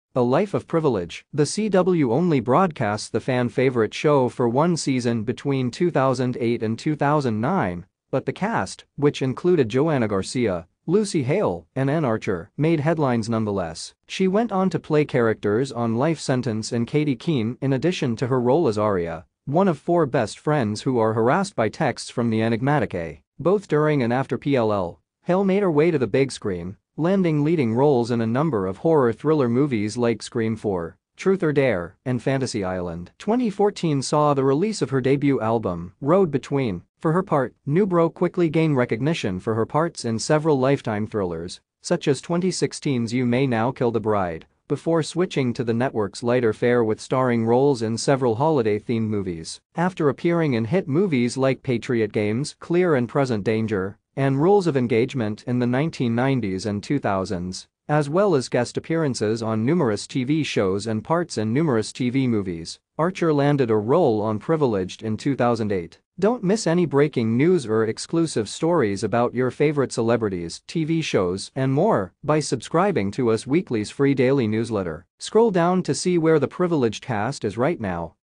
Where Are the Cast Members of "Privileged" Now? Ashley Newbrough, Lucy Hale, Joanna Garcia, and...
A life of privilege! The CW only broadcast the fan-favorite show for one season between 2008 and 2009, but the cast, which included JoAnna Garcia, Lucy Hale, and Anne Archer, made headlines nonetheless.